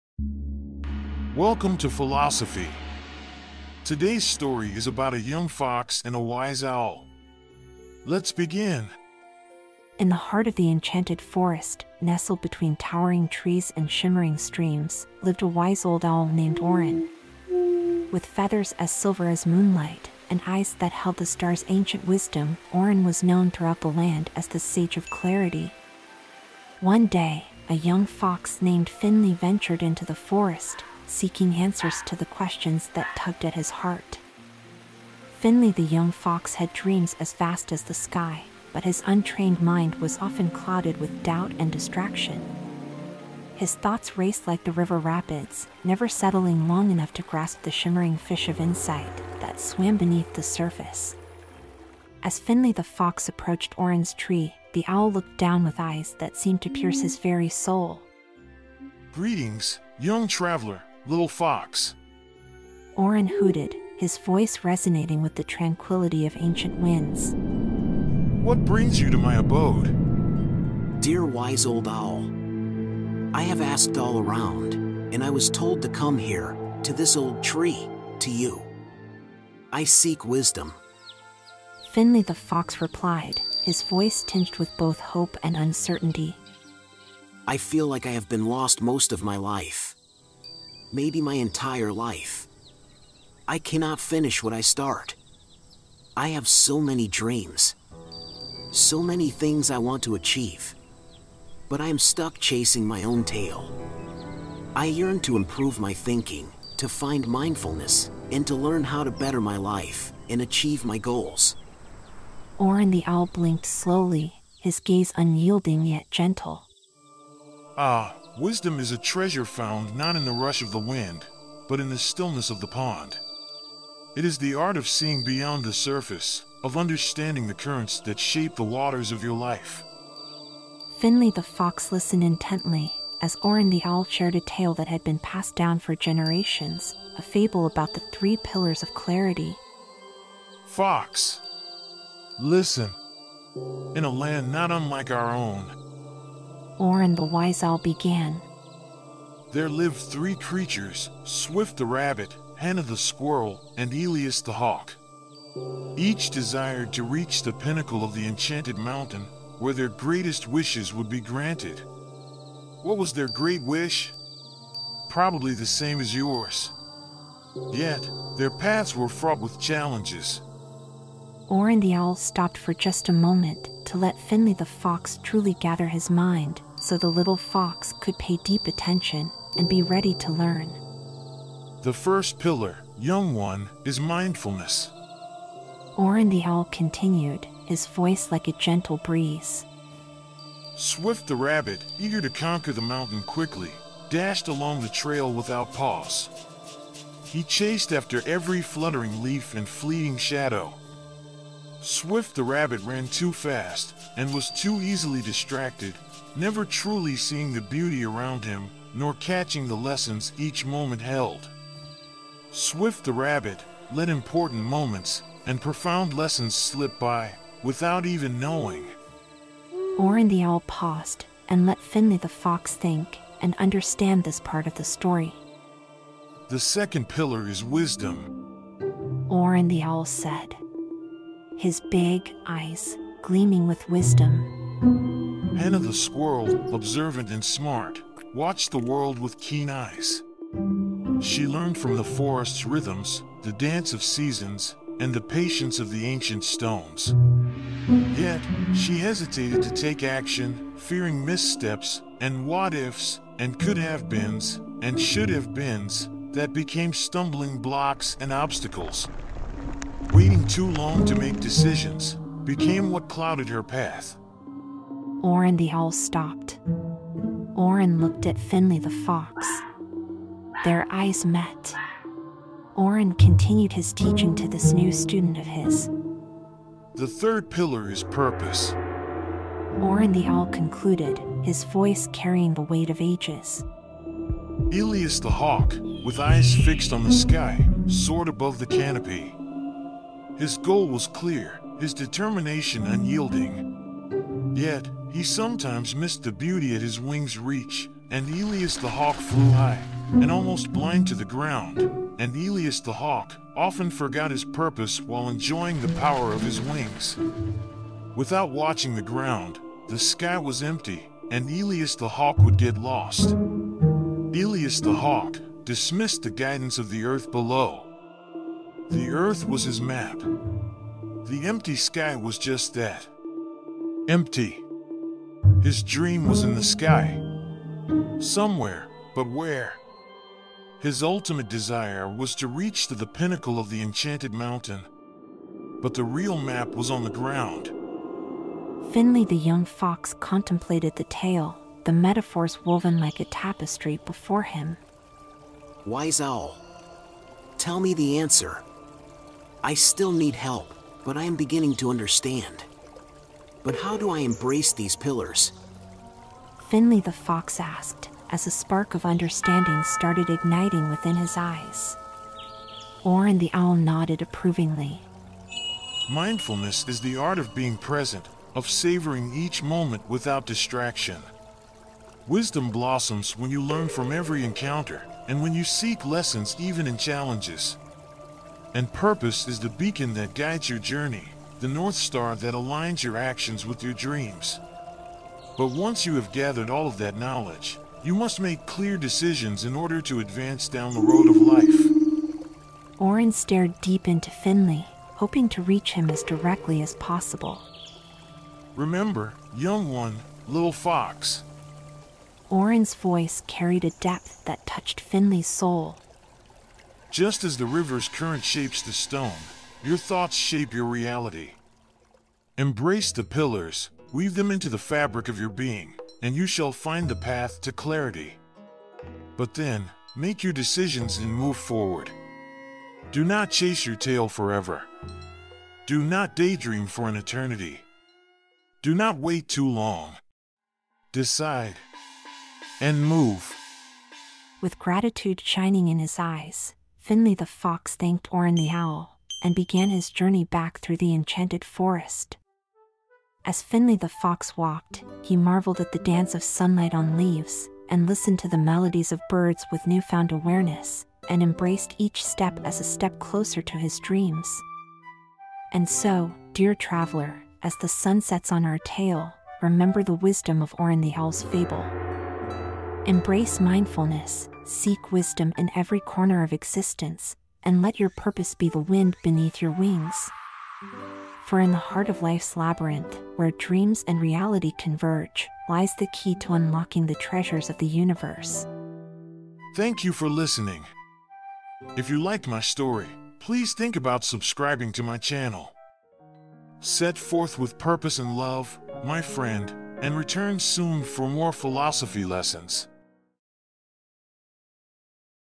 3 Signs You’re Ruining Golden Opportunities! — Don’t Regret Your Life Choices! — A Story in Anime
Welcome to Philosophy. Today’s story is about a young fox and a wise owl. Let’s begin…

In the heart of the Enchanted Forest, nestled between towering trees and shimmering streams, lived a wise old owl named Orin.

With feathers as silver as moonlight, and eyes that held the stars' ancient wisdom, Orin was known throughout the land as the “Sage of Clarity”.

One day, a young fox named Finley ventured into the forest, seeking answers to the questions that tugged at his heart.

Finley the young fox had dreams as vast as the sky, but his untrained mind was often clouded with doubt and distraction.

His thoughts raced like the river rapids, never settling long enough to grasp the shimmering fish of insight… that swam beneath the surface.

As Finley the fox approached Orin's tree, the owl looked down with eyes that seemed to pierce his very soul.

"Greetings, young traveler, little fox…"

Orin hooted, his voice resonating with the tranquility of ancient winds.

"What brings you to my abode?"

"Dear wise old owl. I have asked all around, and I was told to come here, to this old tree, to you. I seek wisdom."

Finley the fox replied, his voice tinged with both hope and uncertainty.

"I feel like I have been lost most of my life. Maybe my entire life. I cannot finish what I start. I have so many dreams. So many things I want to achieve. But I am stuck chasing my own tail. I yearn to improve my thinking, to find mindfulness, and to learn how to better my life, and achieve my goals."

Orin the owl blinked slowly, his gaze unyielding yet gentle.

"Ah, wisdom is a treasure found not in the rush of the wind, but in the stillness of the pond. It is the art of seeing beyond the surface, of understanding the currents that shape the waters of your life."

Finley the fox listened intently, as Orin the owl shared a tale that had been passed down for generations, a fable about the “Three Pillars of Clarity”.

"Fox. Listen. In a land not unlike our own…"

Orin the wise owl began…

"There lived three creatures: Swift the Rabbit, Hana the Squirrel, and Aelius the Hawk. Each desired to reach the pinnacle of the Enchanted Mountain, where their greatest wishes would be granted. What was their great wish? Probably the same as yours. Yet, their paths were fraught with challenges."

Orin the owl stopped for just a moment, to let Finley the fox truly gather his mind, so the little fox could pay deep attention, and be ready to learn.

"The first pillar, young one, is mindfulness."

Orin the owl continued, his voice like a gentle breeze.

"Swift the Rabbit, eager to conquer the mountain quickly, dashed along the trail without pause. He chased after every fluttering leaf and fleeting shadow. Swift the Rabbit ran too fast, and was too easily distracted, never truly seeing the beauty around him, nor catching the lessons each moment held. Swift the Rabbit, let important moments, and profound lessons slip by, without even knowing."

Orin the owl paused, and let Finley the fox think, and understand this part of the story.

"The second pillar is wisdom…"

Orin the owl said. His big — eyes — gleaming with wisdom.

"Hana the Squirrel, observant and smart, watched the world with keen eyes. She learned from the forest's rhythms, the dance of seasons, and the patience of the ancient stones. Yet, she hesitated to take action, fearing missteps, and what-ifs, and could-have-beens, and should-have beens, that became stumbling blocks and obstacles. Waiting too long to make decisions, became what clouded her path."

Orin the owl stopped. Orin looked at Finley the fox. Their eyes met. Orin continued his teaching to this new student of his…

"The third pillar is purpose…"

Orin the owl concluded, his voice carrying the weight of ages.

"Aelius the Hawk, with eyes fixed on the sky, soared above the canopy. His goal was clear, his determination unyielding. Yet, he sometimes missed the beauty at his wings' reach, and Aelius the Hawk flew high, and almost blind to the ground, and Aelius the Hawk, often forgot his purpose while enjoying the power of his wings. Without watching the ground, the sky was empty, and Aelius the Hawk would get lost. Aelius the Hawk, dismissed the guidance of the earth below. The Earth was his map. The empty sky was just that. Empty. His dream was in the sky. Somewhere, but where. His ultimate desire was to reach the the pinnacle of the Enchanted Mountain. But the real map was on the ground."

Finley the young fox contemplated the tale, the metaphors woven like a tapestry before him.

"Wise owl… Tell me the answer… I still need help, but I am beginning to understand. But how do I embrace these pillars?"

Finley the fox asked, as a spark of understanding started igniting within his eyes.

Orin the owl nodded approvingly.

"Mindfulness is the art of being present, of savoring each moment without distraction.“